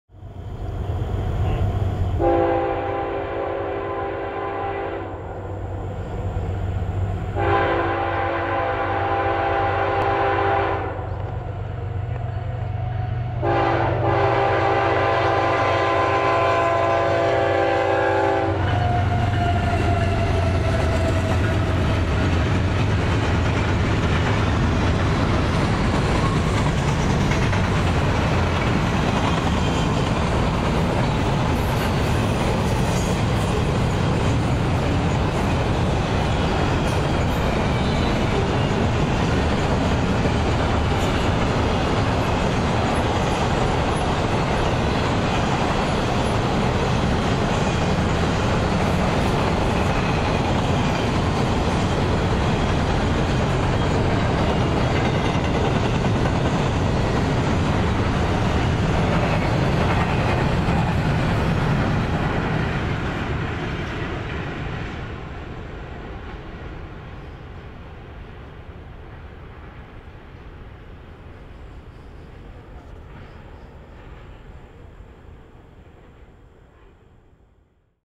(34) A Friendly Wave From CSX @ Westfield Oct. 2 2014
A friendly wave from the conductor of this west bound CSX double stack container train as it passes Westfield, New York. October 2 2014.